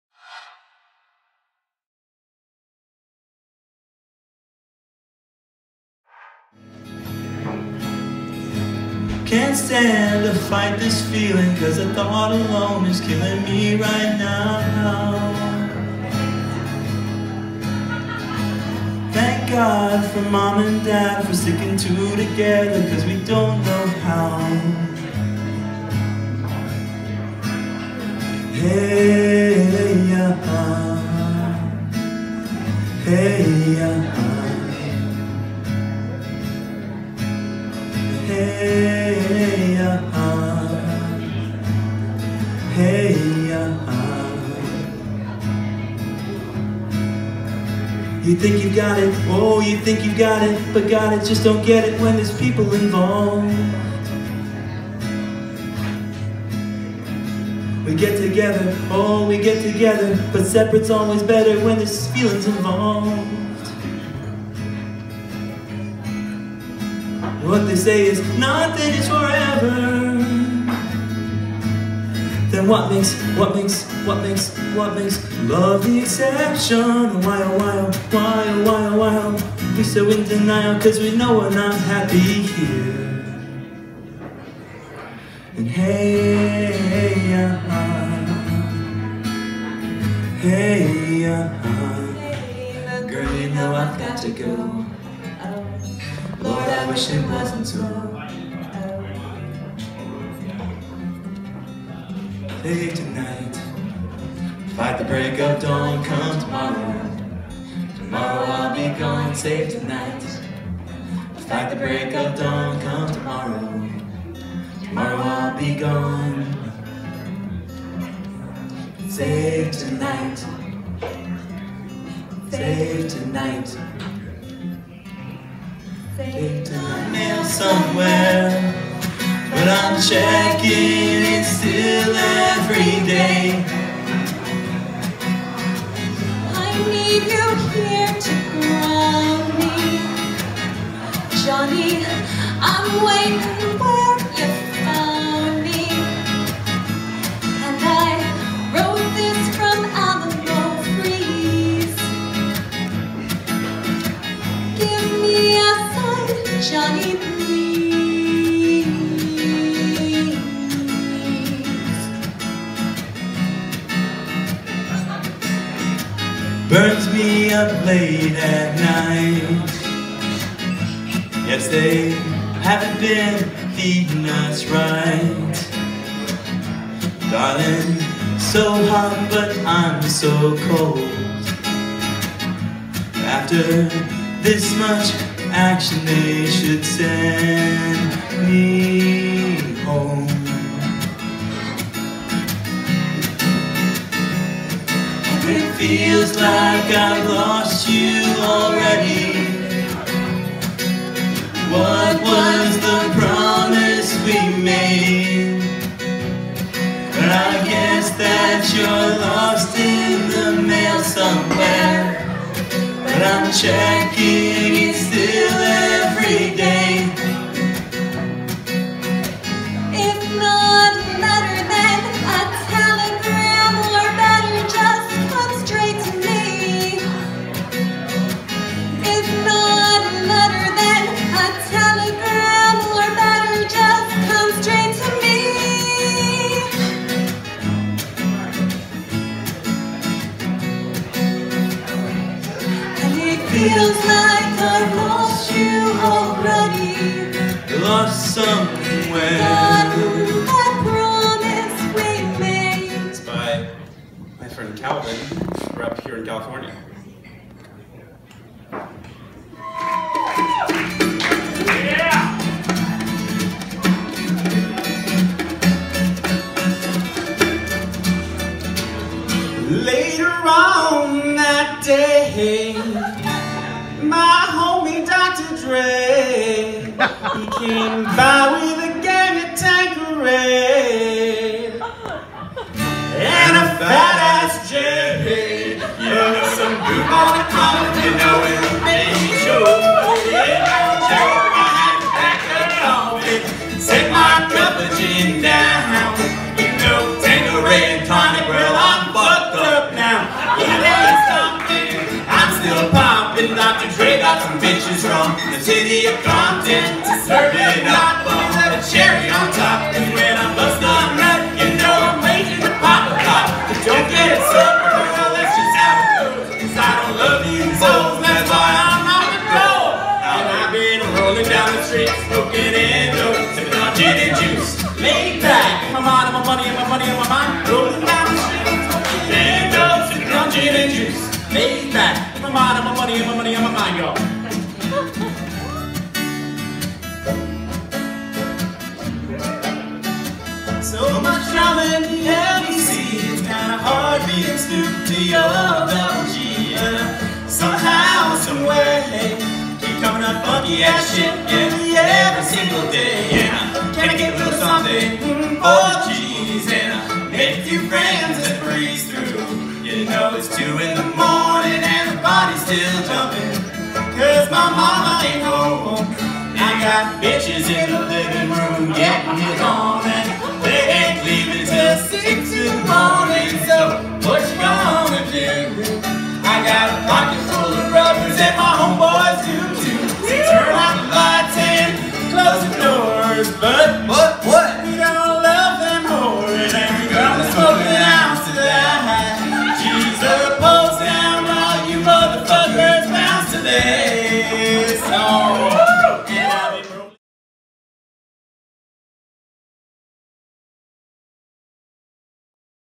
CDS First Show
Craig plays his first show ever at Sweetwater in Mill Valley